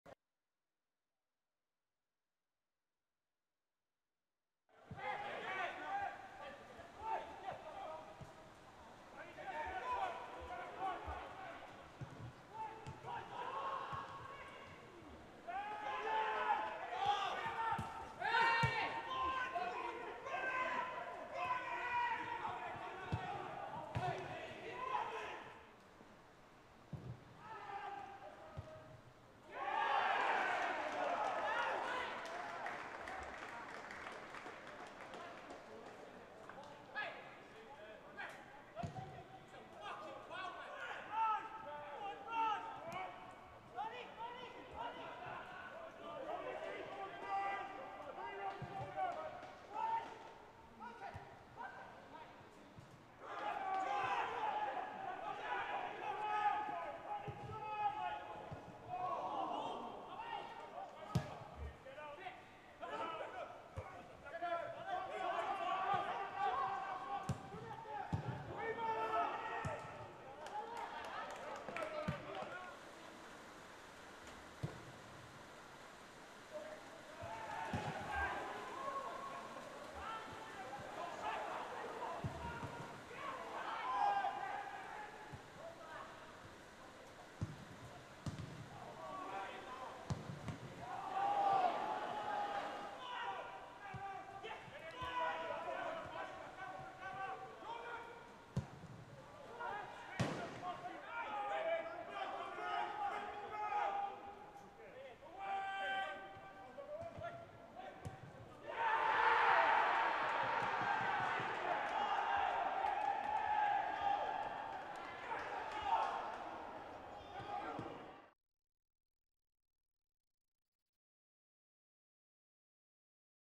Town 1-1 Gillingham | 2020/21 Highlights
Short highlights of Town's 1-1 draw with Gillingham.